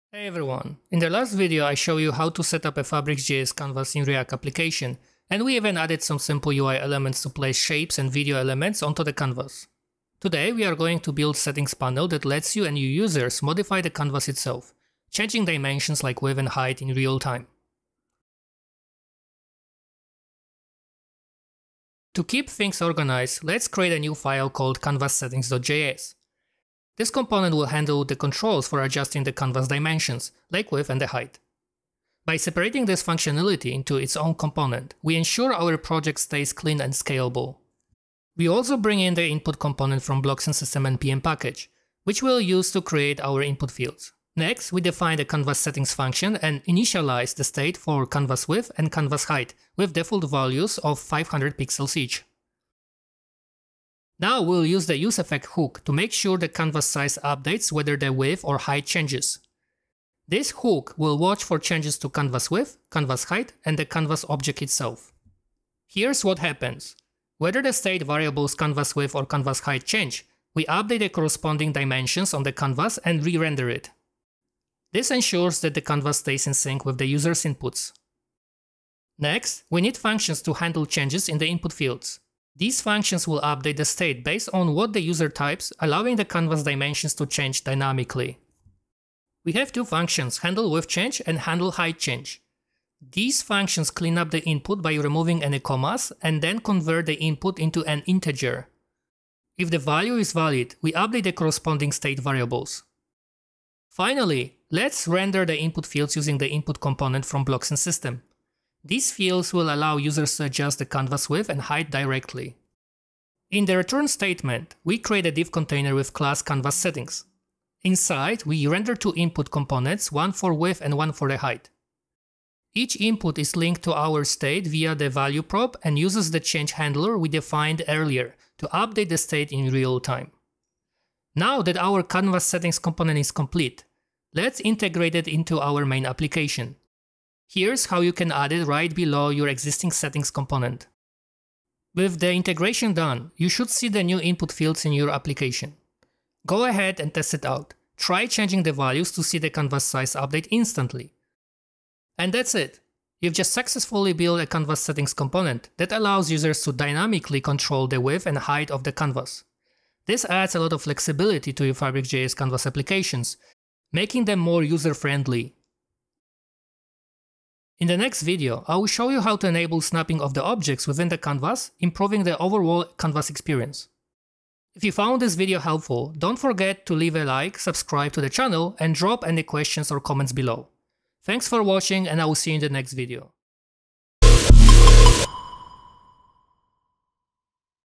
FabricJs 6 and React Tutorial | Change Canvas Size - Adjust Height and Width of Canvas HTML - Part 4
In this video, we dive deeper into building dynamic canvas applications with Fabric.js 6 and React. Today, we're creating a settings panel that allows real-time adjustments to your canvas dimensions, like width and height. Perfect for making your Fabric.js projects more flexible and user-friendly!

🔧 What you'll learn:

👉 Setting up the CanvasSettings component.
👉 Updating canvas dimensions in real-time with useEffect hook.
👉 Handling user inputs to modify the canvas.

By the end, you'll have a fully functional settings panel integrated into your React app, making it easier to control your canvas size dynamically.

👉 Next Up: Enabling object snapping within the canvas for a better user experience.

Check out the previous video: FabricJs 6 and React Tutorial | Add, Control, and Export Videos in Canvas HTML - Part 3